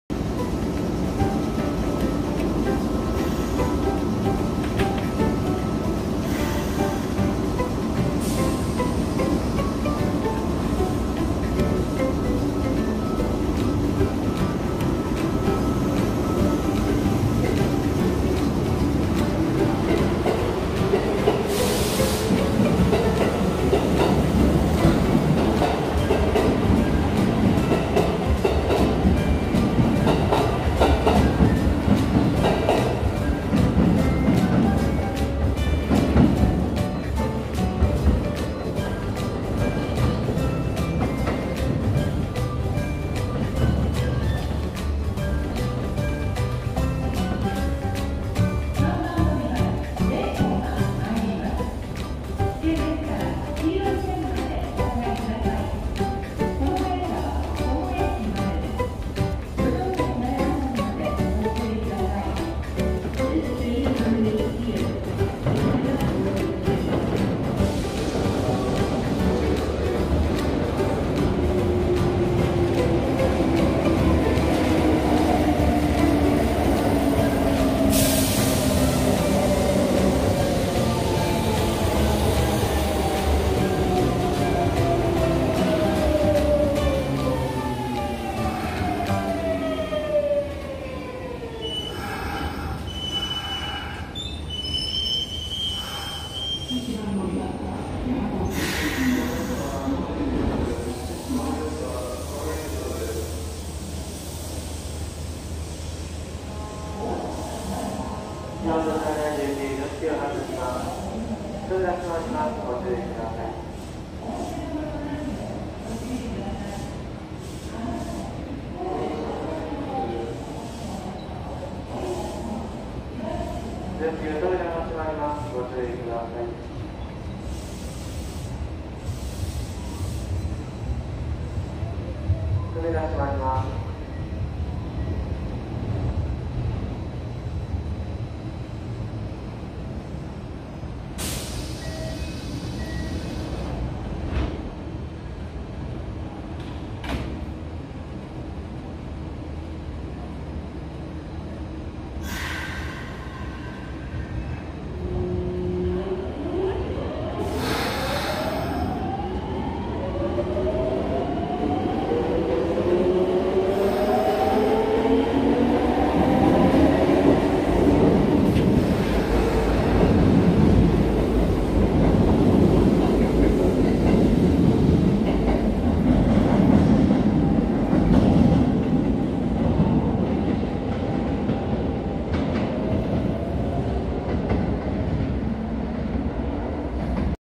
師走の第2土曜日 part⑮【大阪難波駅/発着風景】
師走の第2土曜日part⑮【大阪難波/発着風景】

②・③・①番線での発着風景の「ヒトコマ」です。

⏹️撮影日 2019年(令和元年)12月14日(土)22時10分～13分頃